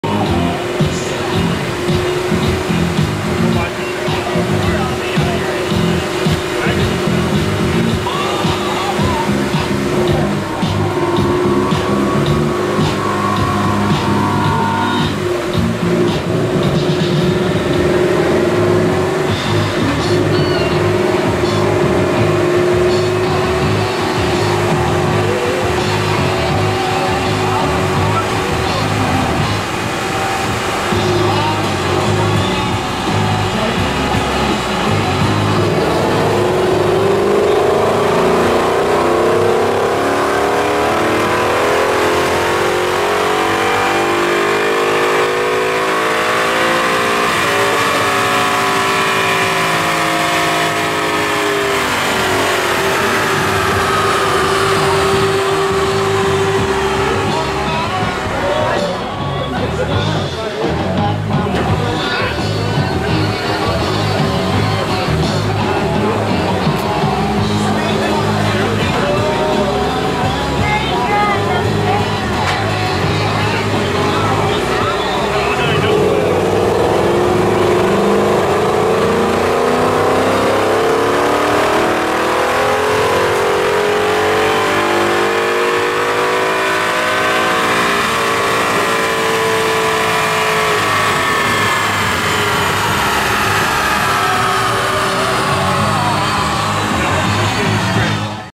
389 hp 2011 Ford Mustang GT - Michael Morrissey
Auto Plaza Ford Dyno Day, May 11, 2013 in DeSoto, MO. Michael Morrissey's 2011 Ford Mustang GT on the Track Lab Dyno.  Max hp = 389.34. Max ft lbs = n/a.